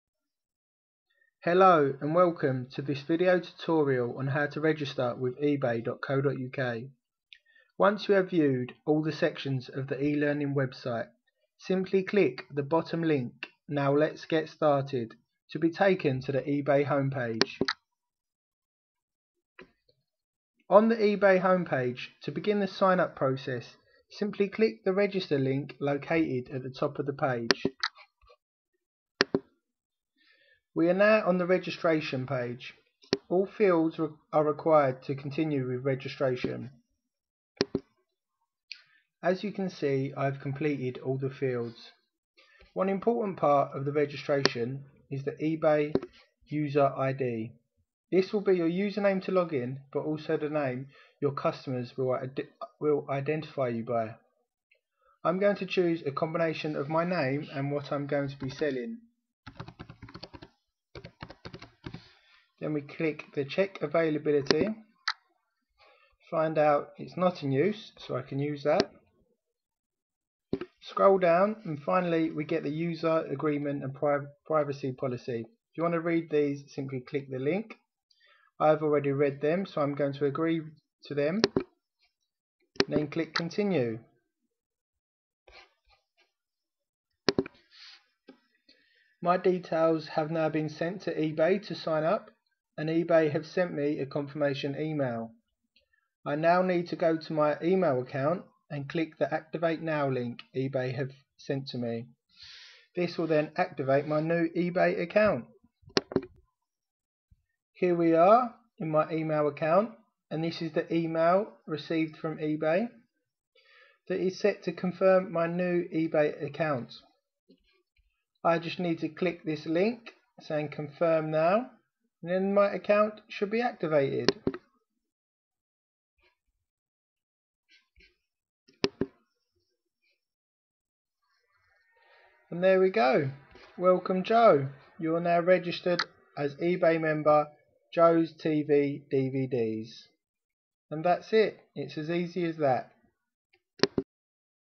E-Learning for eBay Registration eBay Seller Tutorial Guide - Make Money on eBay
2009 Uni Project on E-Learning

E-Learning for eBay. How to Register an eBay Account